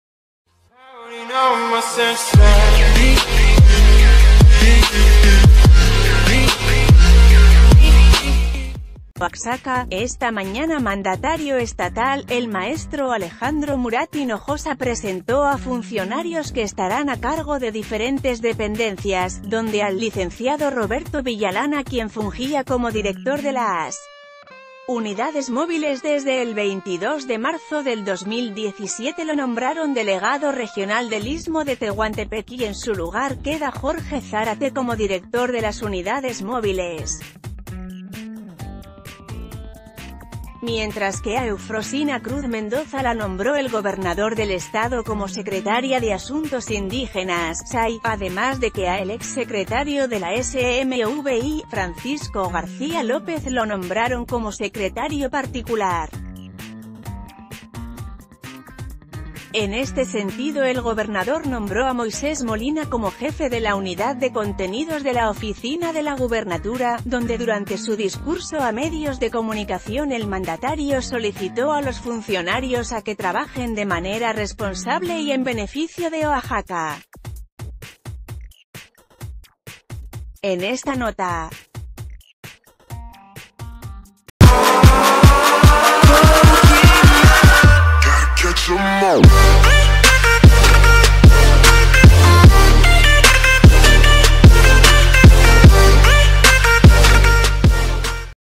Alejandro Murat Hinojosa realiza movimientos en su gabinete
Oaxaca.- Esta mañana Mandatario Estatal, el Maestro Alejandro Murat Hinojosa presentó a funcionarios que estarán a cargo de diferentes dependencias, donde al Licenciado Roberto Villalana quien fungía como director de las unidades móviles desde el 22 de marzo del 2017 lo nombraron delegado regional d...